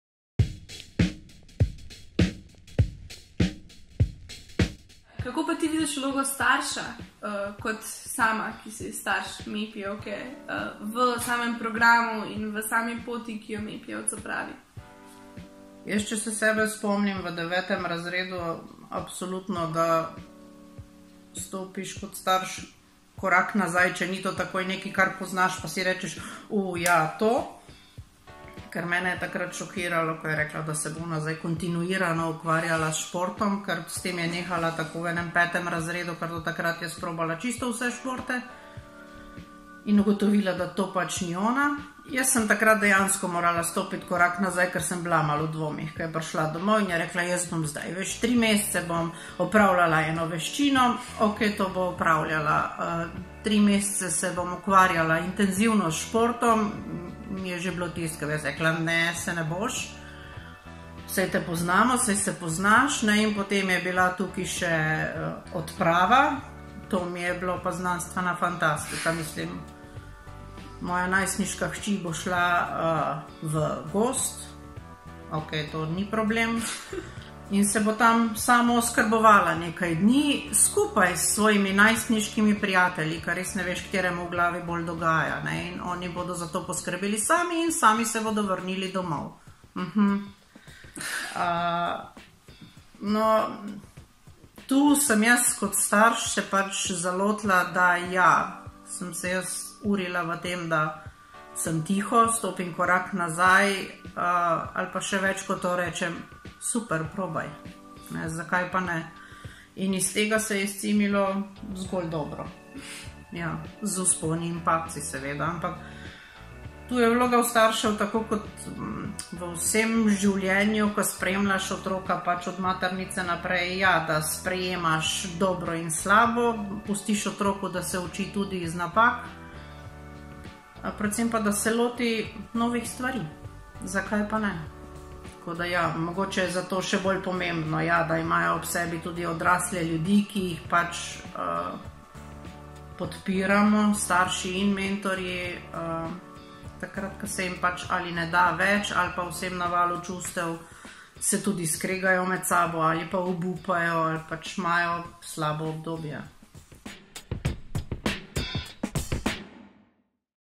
Vloga starša v MEPI
Hana Bujanović Kokot je MEPI zlatnica, dijakinja 4. letnika Gimnazije Kranj, ki se je v programu vključila v 9. razredu osnovne šole. Na celotni poti jo je spremljala njena mami, specialna pedagoginja Helena Kokot.

V prispevku mama Helena deli svoj pogled na vlogo starša v programu MEPI.